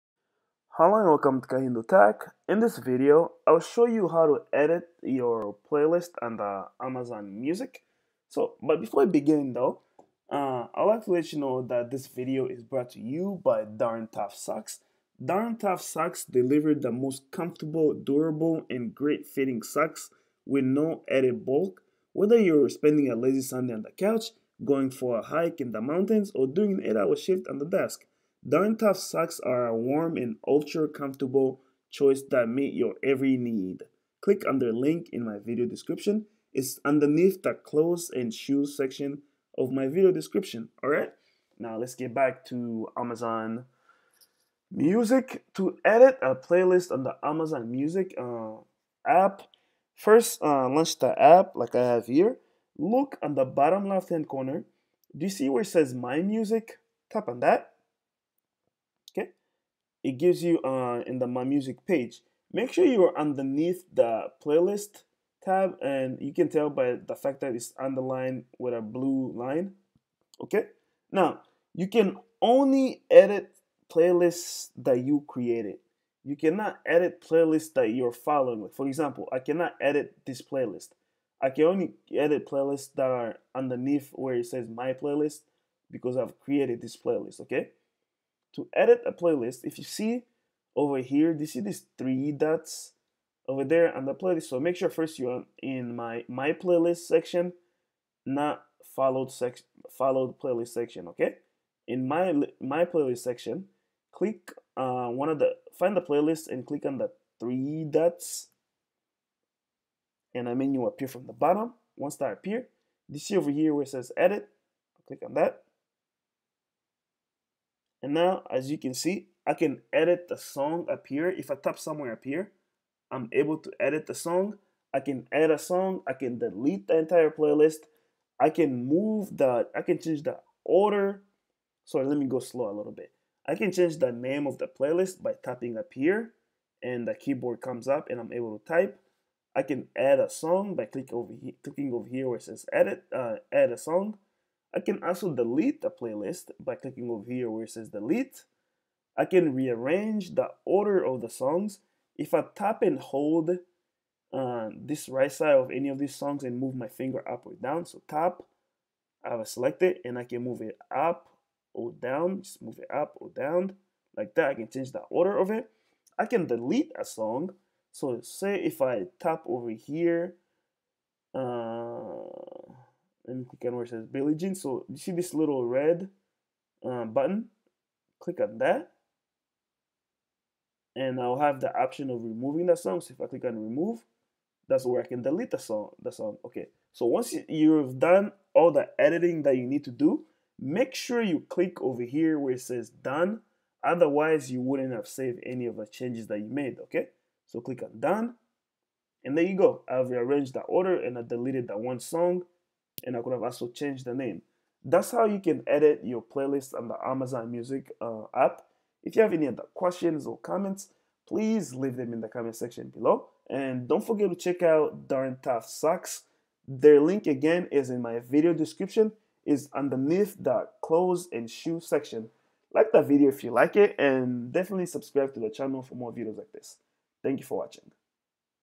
How To Edit Playlist Amazon Music
This video shows How To Edit Playlist Amazon Music.

Amazon Deals
- Amazon Music Unlimited 3-Months For Only $0.99 (Ends 12/30/2019)
- Amazon Prime Music Free 30-Day Trial